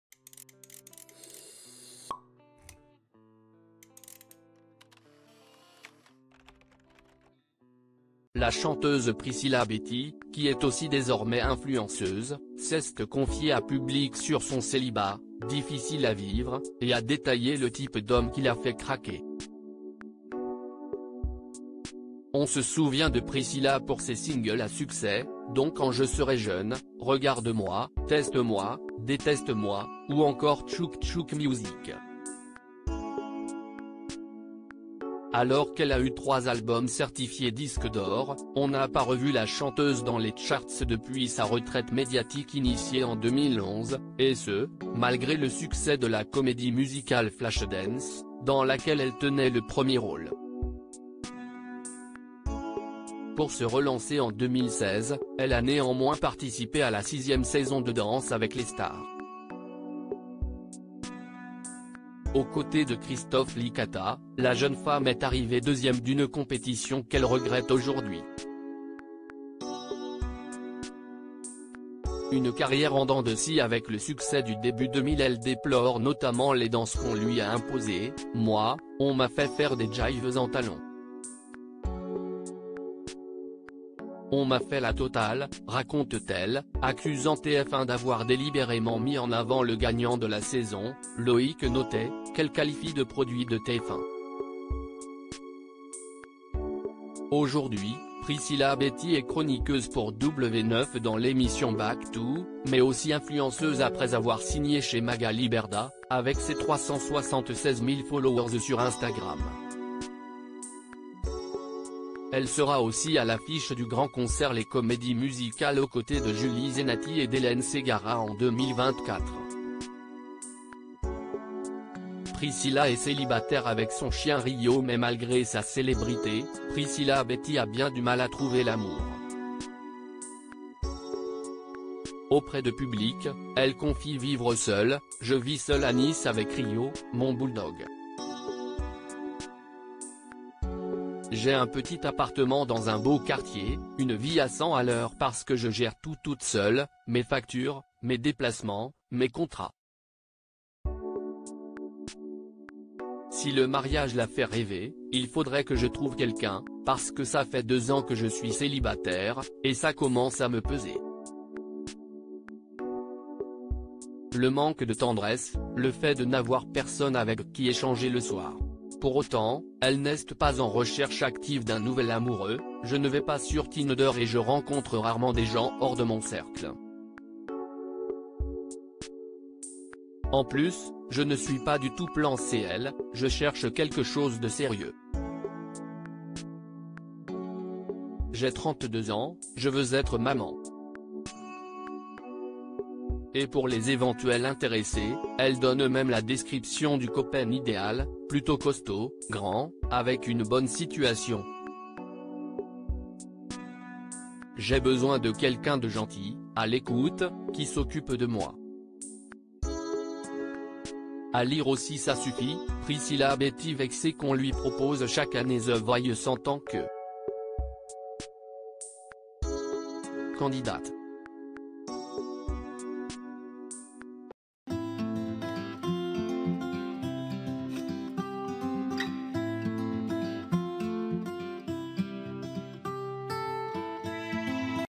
"Il faudrait que je trouve quelqu'un" : Priscilla Betti cash sur son célibat qui "commence à lui pes
"Il faudrait que je trouve quelqu'un" : Priscilla Betti cash sur son célibat qui "commence à lui peser" et sa quête de l'âme sœur